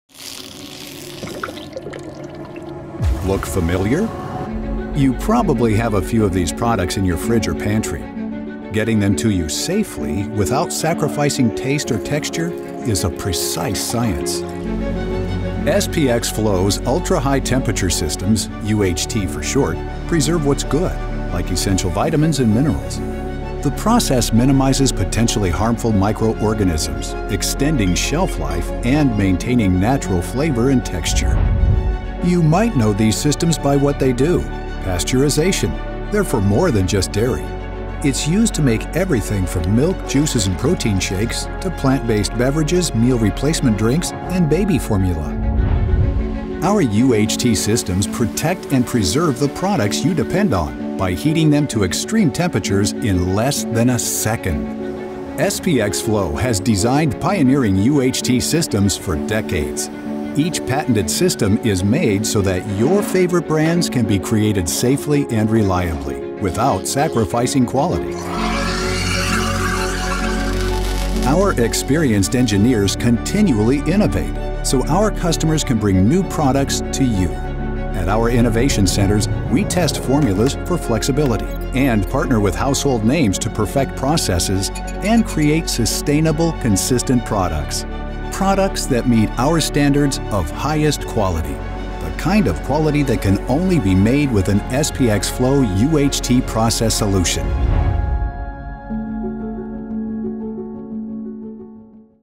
SPX FLOW - UHT System Product Video
A short overview of SPX FLOW’s ultra-high temperature (UHT) systems. Its shows how we help products safely get to your fridge and freezer without sacrificing taste or texture. (Originally broadcast during Investors’ Day 2021)